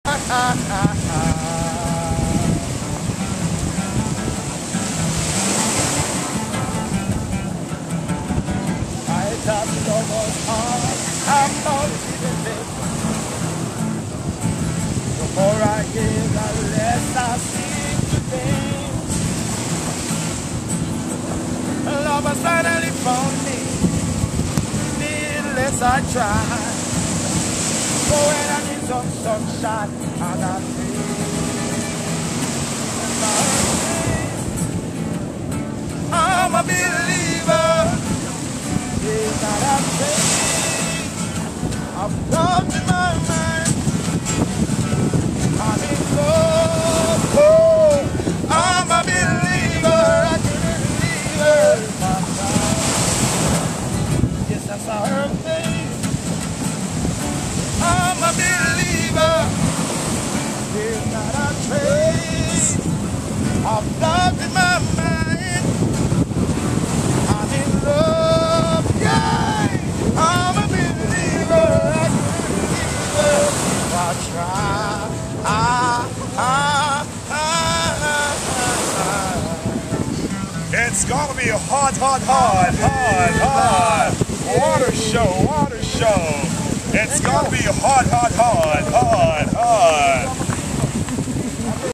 Couples Negril Cigar Guy
Cigarettes and Cigars! It's gonna be hot, hot, hot! Loved this guy! Couples, Negril has vendors on the beach, but they don't bother you. Just say no thanks, and they leave you alone.